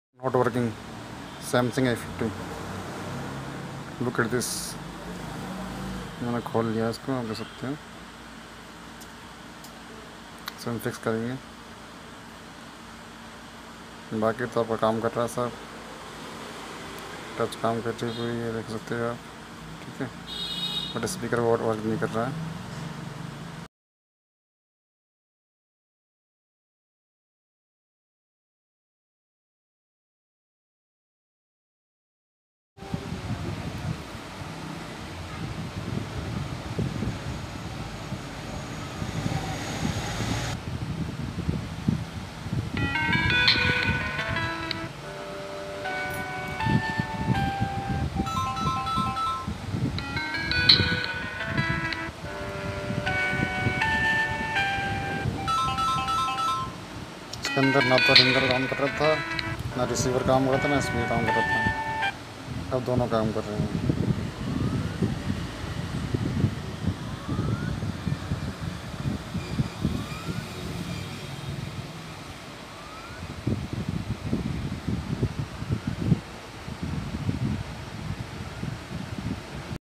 सैमसंग A50 स्पीकर और रिसीवर को ठीक कैसे करे Samsung A50 Media Speaker , Receiver Not working Fix.
Samsung A50 Media Speaker , Receiver Not working Fix.
Latest Problem in Samsung Mobile Phones , a50, a20, a30 all a series Mobile phone .